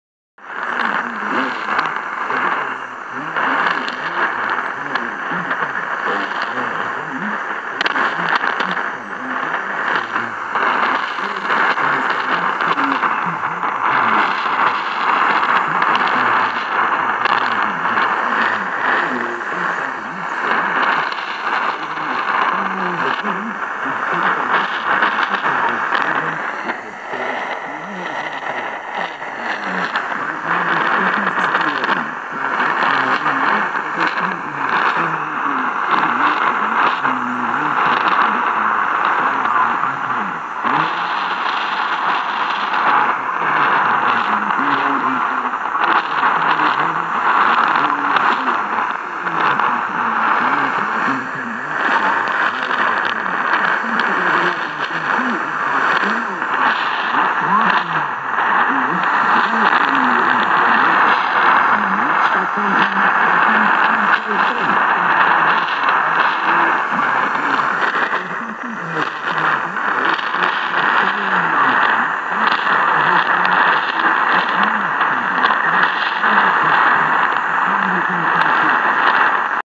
3325 kHz NBC Bougaiville (Papua New Guinea 90 meters band)
3325 kHz NBC Bougaiville (Papua New Guinea 90 meters band) Local Comments 09:46 UTC 08 Mar. 2019 RX: Degen DE 1103 Antenna: Telescopic
Daniel Wyllyans Nova Xavantina MT Brazil